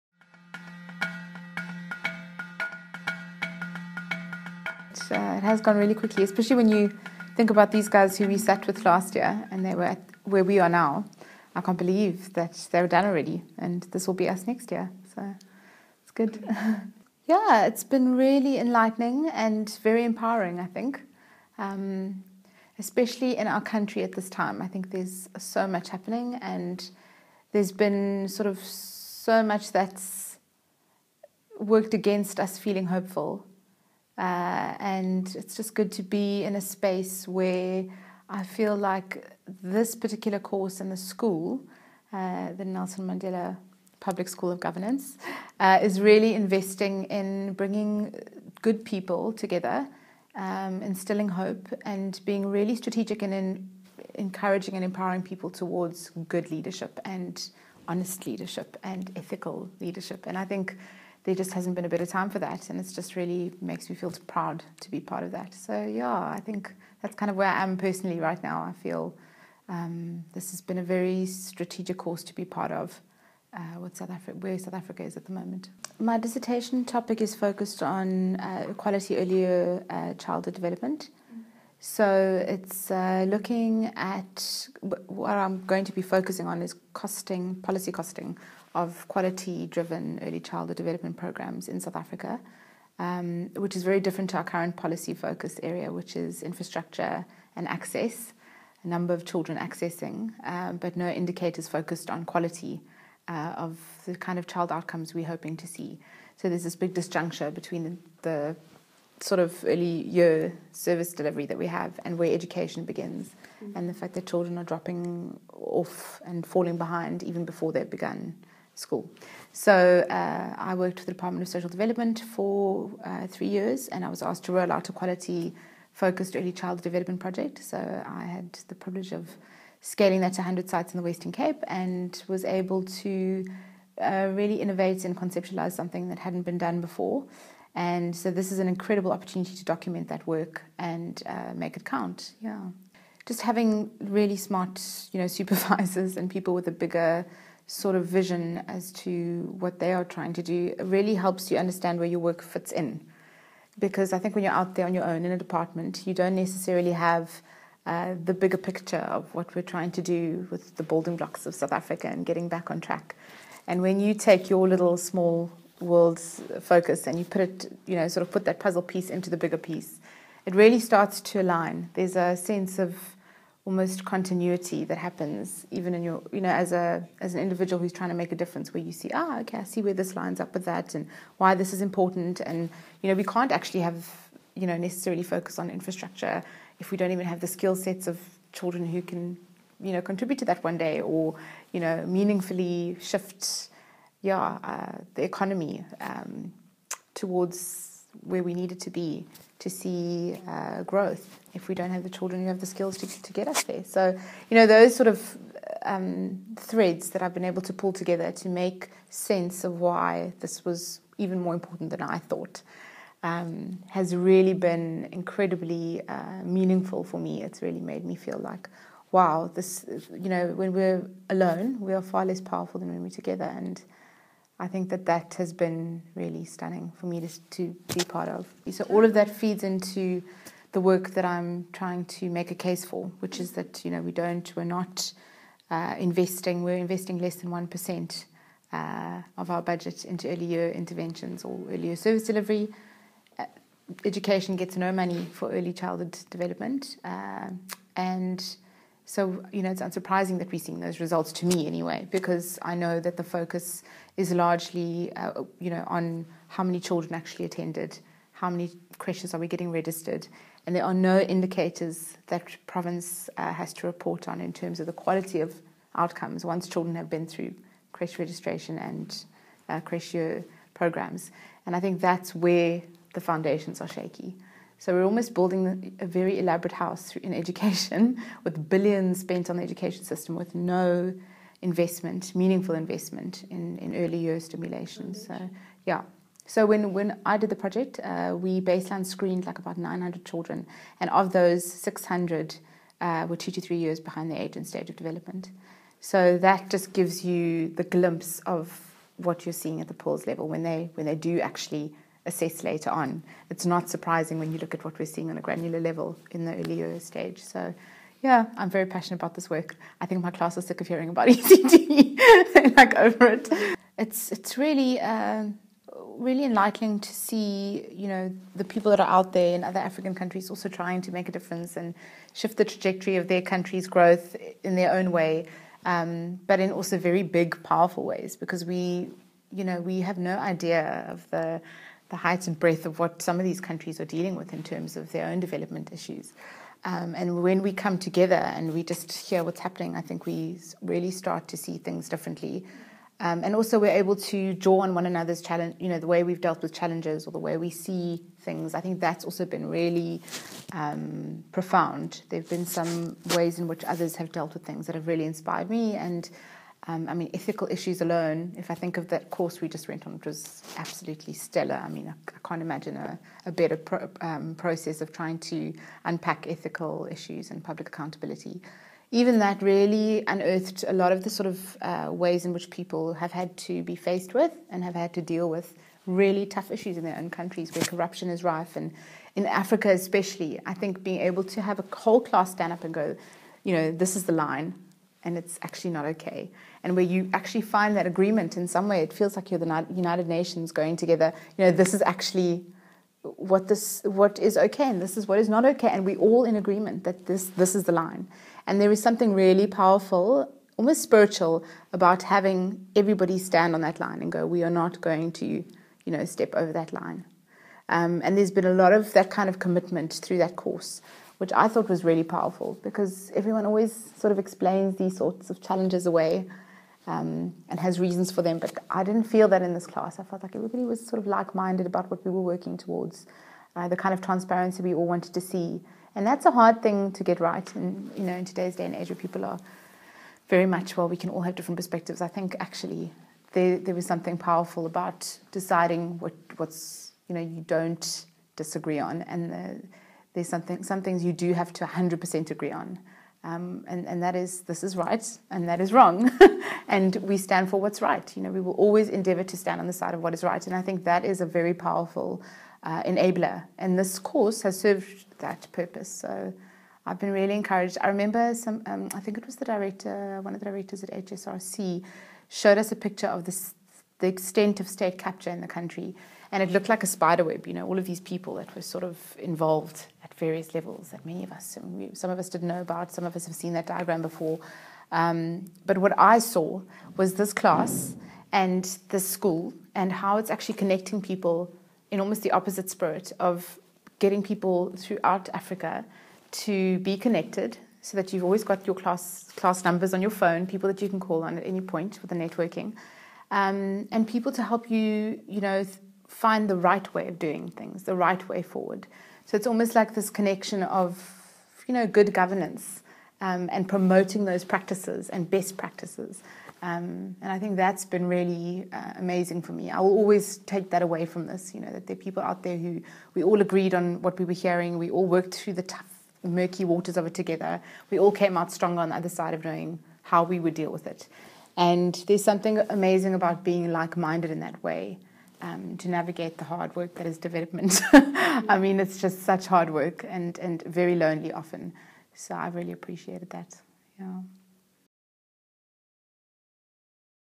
Master's Exit Interview - Kamesh Flynn
The Mandela School Master's in Development Policy and Practice brings together professionals from across the African continent. Here, individuals from the class of 2017 speak about their time in the programme.